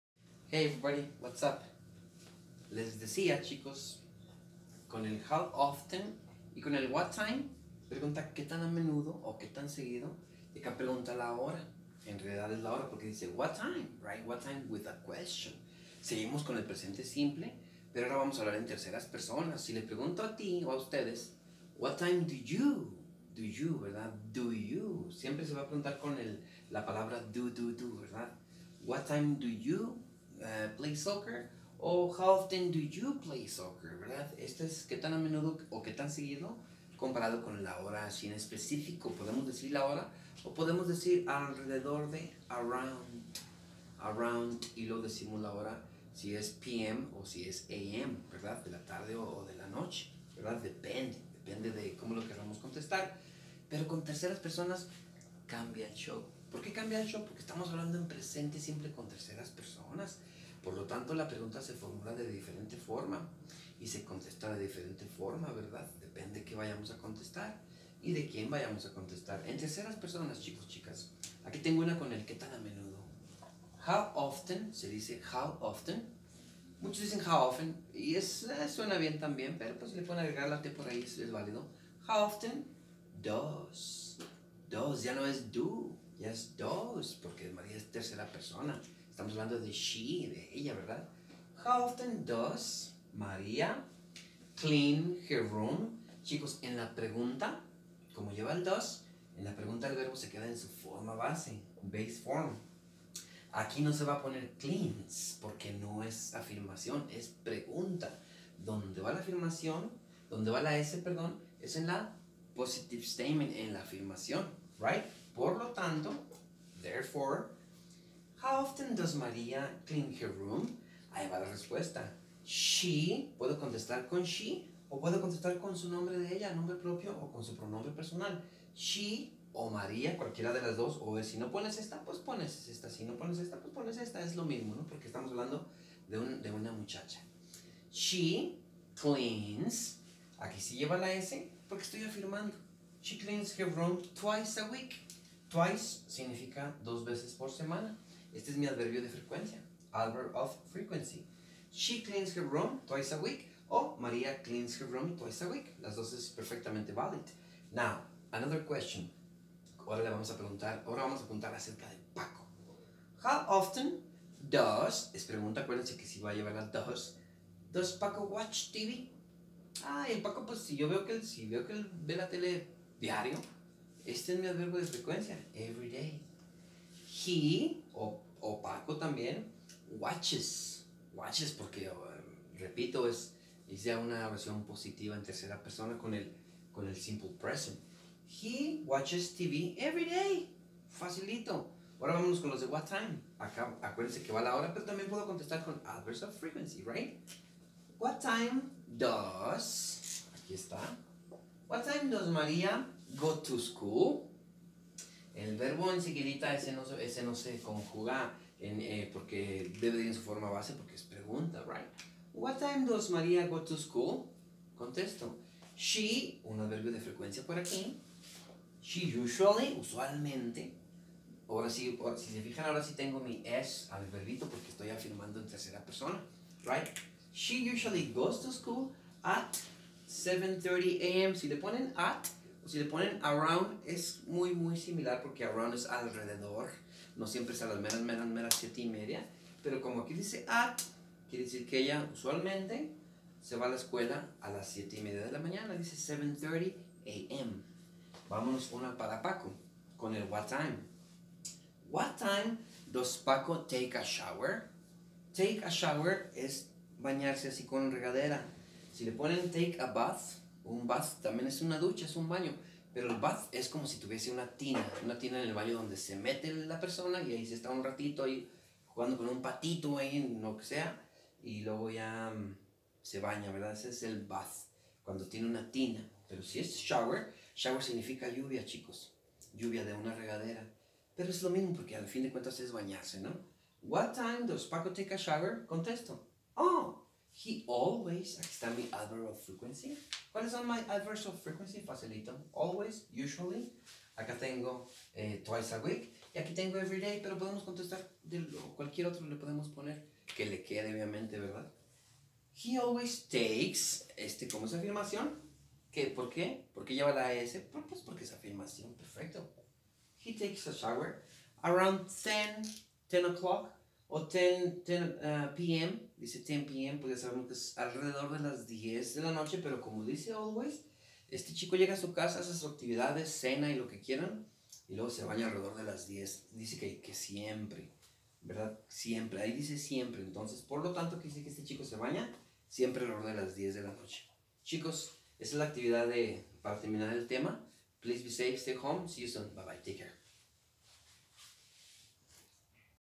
LVL 5 HOW OFTEN? WHAT TIME? Part 2 Third Person
How to answer HOW OFTEN and WHAT TIME questions with Adverbs of Frequency. Third Person